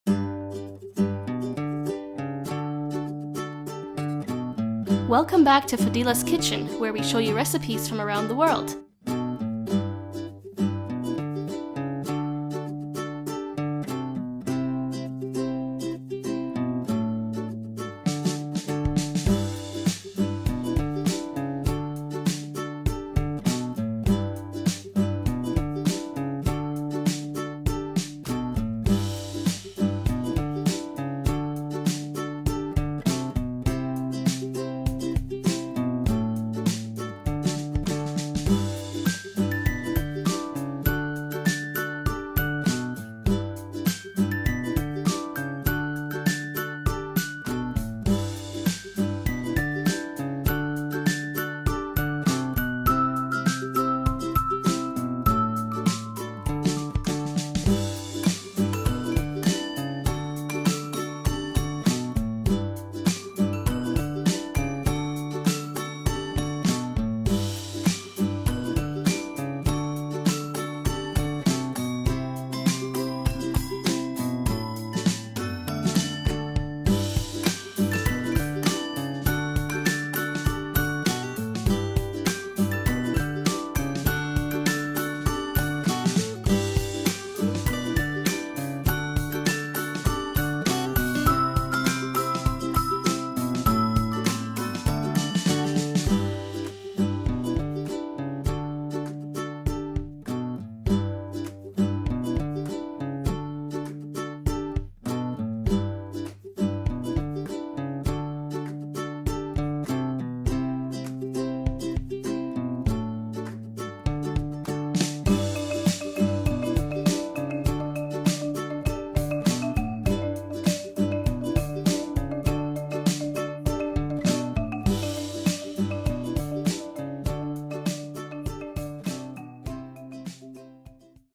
Up Your Rice Game - Try This Today!
Rice, water, onion ,salt and oil
And you have yummy delicious Rice Recipe
Just try it and you will thanks me 💜

Full Ingredients:
- 1.5 cup of jasmine rice
- 2 cups water
- 2 tbsp oil
- 1 tsp salty
- 1 red onion shopped
- 2 garlic cloves minced

Craving more?

Fadila